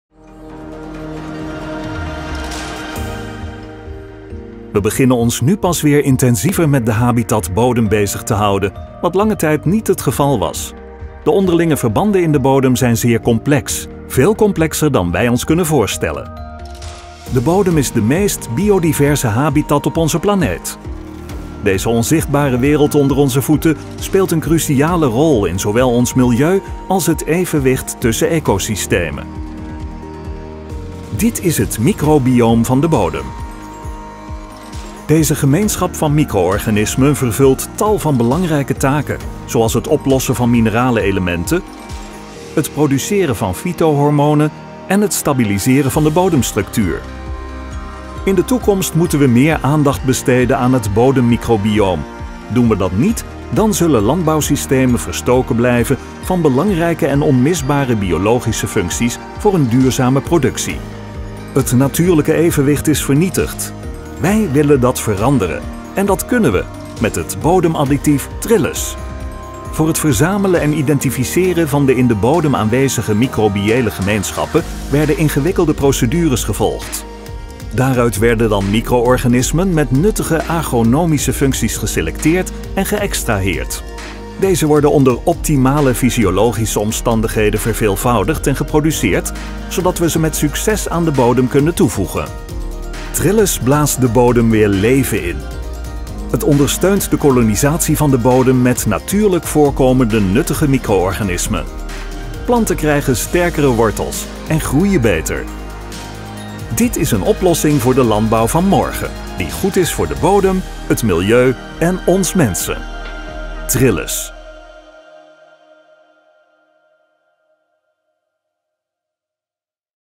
Bodemverbetering / herkolonisatie van de bodem met micro-organismen - Trillus bodemtoevoegingsmiddel
Het biologische bodemadditief Trillus helpt landbouwers de bodemvruchtbaarheid en de opbrengstcapaciteit van hun land te bevorderen. Op die manier kunnen nuttige micro-organismen met succes aan de bodem worden toegevoegd. Meer informatie

De instandhouding en de verhoging van de bodemvruchtbaarheid is de basis voor een succesvolle akkerbouw, nu en in de toekomst. Deze benaderingen worden ook gevolgd in de regeneratieve landbouw in het bijzonder. Het is de moeite waard meer aandacht te besteden aan onze stille helpers in de bodem - de micro-organismen. Zij spelen een cruciale rol, zowel voor ons milieu als voor het evenwicht tussen ecosystemen. We hebben het hier over het bodemmicrobioom.

Micro-organismen (vooral bacteriën en schimmels) vervullen tal van belangrijke functies, zoals het oplossen van minerale elementen, het produceren van fytohormonen en het stabiliseren van de bodemstructuur.
Met het biologische bodemtoevoegingsmiddel Trillus kunnen wij de kolonisatie van de bodem met nuttige micro-organismen bevorderen. De interactie van twee unieke en gepatenteerde stammen (Pseudomonas fluorescens - Ref.: B177-M-03.08 en Trichoderma harzianum - Ref.: B97-M-04.08) bevordert specifiek de nutriëntenopname, de wortelgroei, de mineralisatie van organisch materiaal en de nutriëntenefficiëntie. Planten danken dit met hogere en stabielere opbrengsten. Trillus kan zowel in de conventionele als in de biologische teelt worden gebruikt.